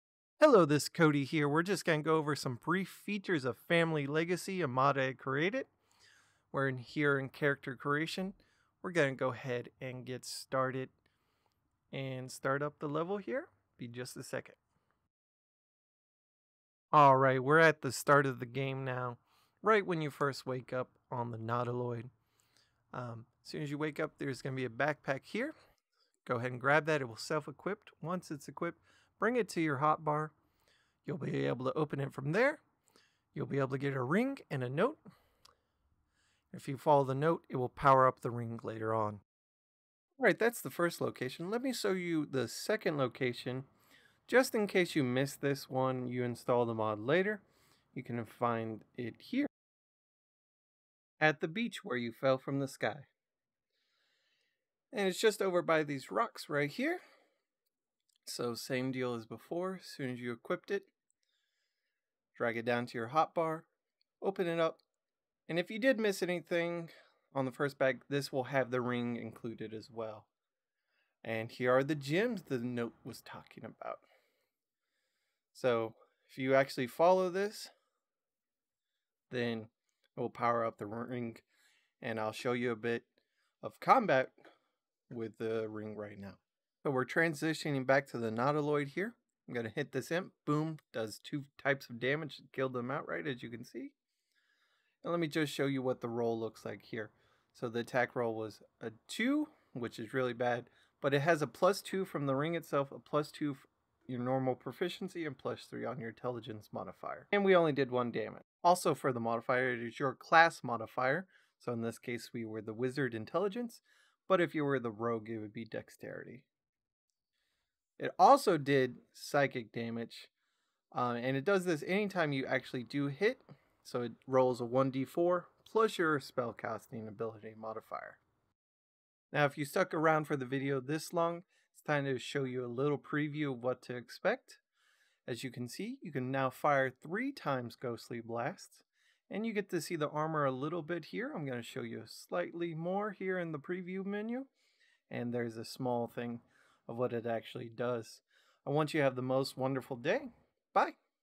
Family Legacy Showcase
This is a small showcase of the Family Legacy Mod that I made showing where to get started and a bit of what it does and what you can expect if you follow the whole quest line.